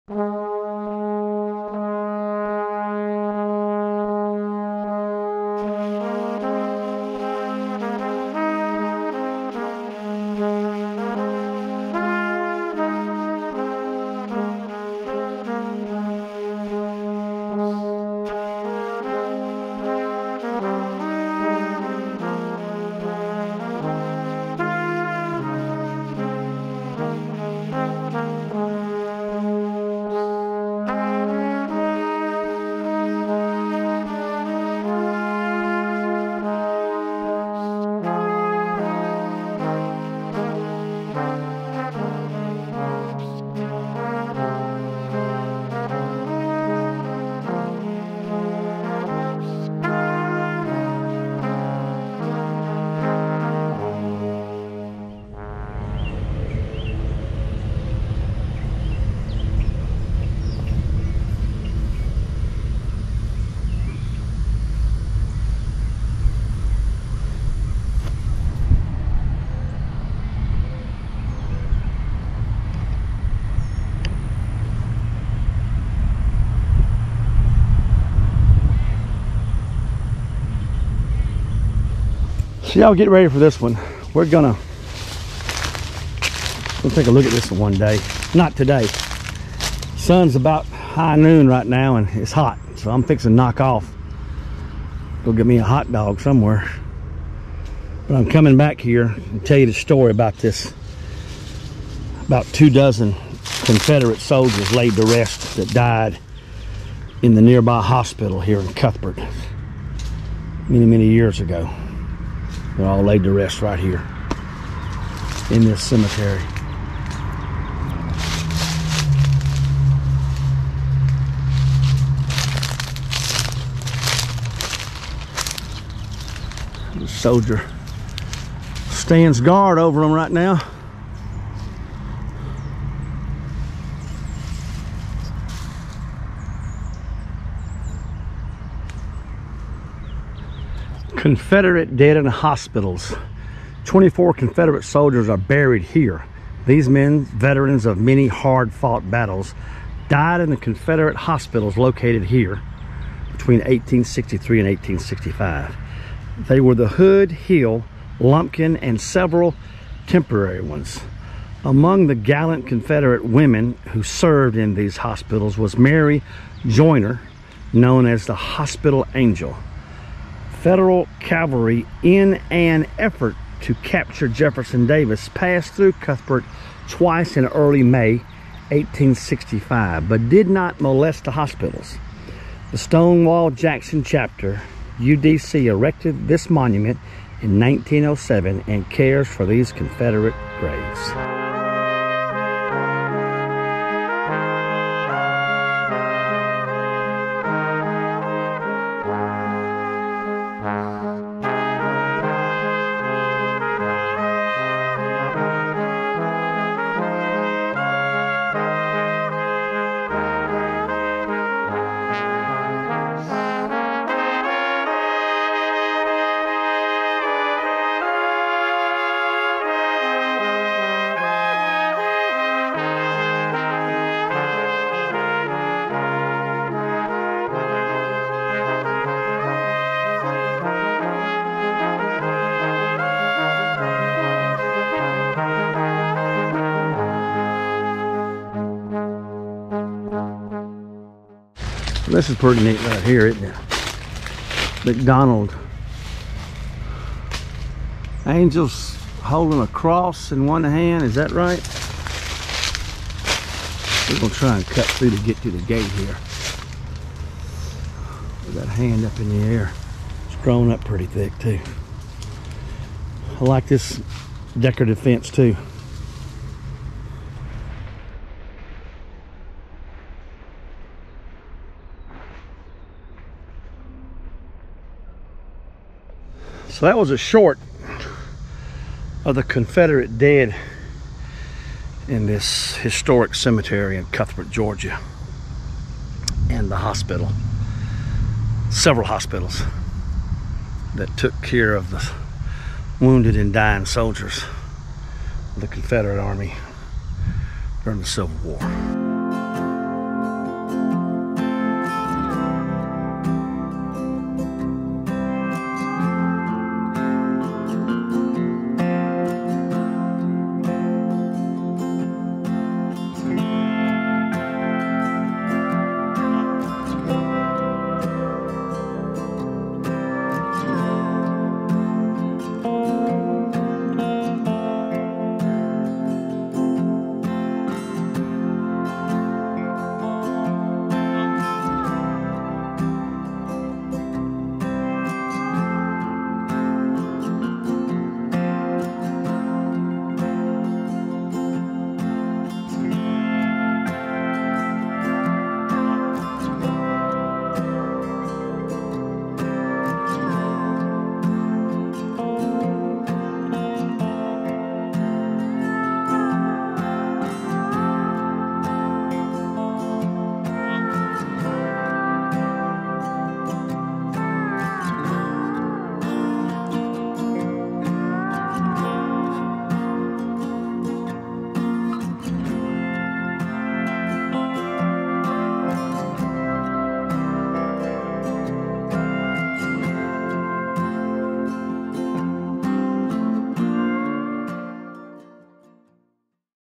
These 24 Confederate Soldiers Died In Hospitals During the Civil War | Cuthbert Georgia
the other robert shows us the graves of 24 confederate soliders who died in rural georgia hospitals during the civil war cuthbert georgia